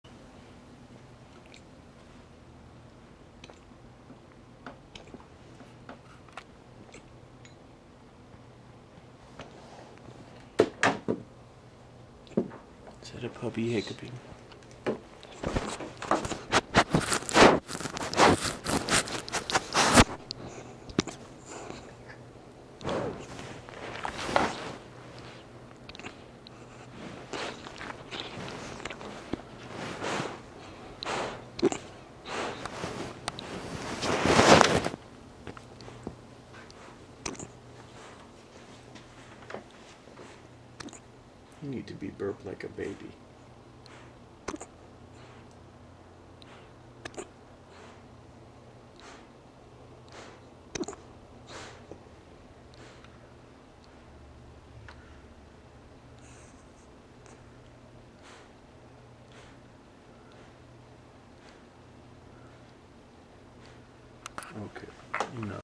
Pit Bull Puppy Attacked by Hiccups!
This adorable puppy has the hiccups!  Panzer Patton is a rescued blue nose, pit bull puppy, 3 mon. at the time of this video.  He was found with severe chemical wounds on his back, and hook worms.  He is swiftly recovering and is much doted upon by his parents, two sets of grandparents, plus extended human and animal family.